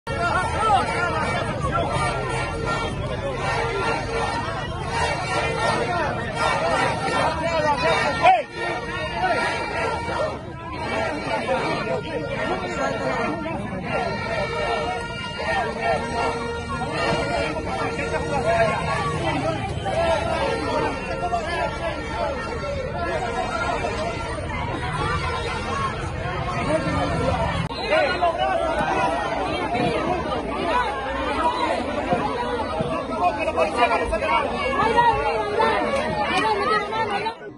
Puñetazos y empujones en marcha del Colegio Médico
Entre puñetazos y empujones se desarrolló la marcha de los médicos hacia el Palacio Nacional. Los galenos quebrantaron el cerco de seguridad y se enfrentaron con agentes policiales que trataban de contenerlos para evitar que se acercaran a la casa de Gobierno.

Al demandar que se cumpla el acuerdo firmado con el Ministerio de Salud y una serie de reivindicaciones, los galenos dijeron que se trata de una “represión.